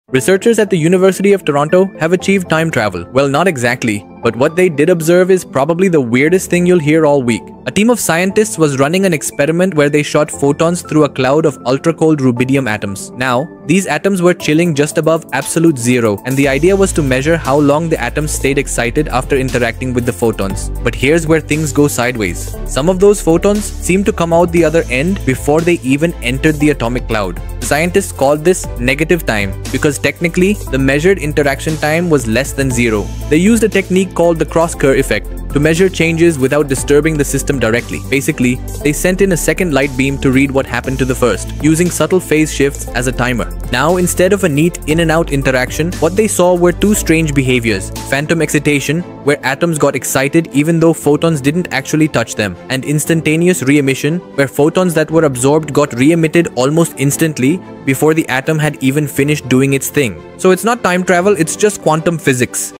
Photons in Negative Time: The Quantum Anomaly
mind blown! 🚀✨ researchers at the university of toronto just observed photons behaving like they're exiting a system before they even enter it 🤯. it's not time travel, but it's definitely weird. 🌌

this phenomenon is all about the strange world of quantum mechanics, where particles can exist in multiple states and behave in crazy ways. 🔮 the researchers used a clever technique called the cross-kerr effect to measure the interaction time, and what they found was astonishing.

no, this doesn't break relativity or allow time travel 🚫. but it does show how time can get fuzzy at the quantum scale ⏱️. events can overlap, blur, and even appear to happen out of order. it's wild to think about what this means for our understanding of reality 🤔.

seven years of research and dozens of tests later, the results are consistent with quantum theory 🔬. it's not a glitch in the matrix, it's just how reality behaves when you zoom all the way in 🔍.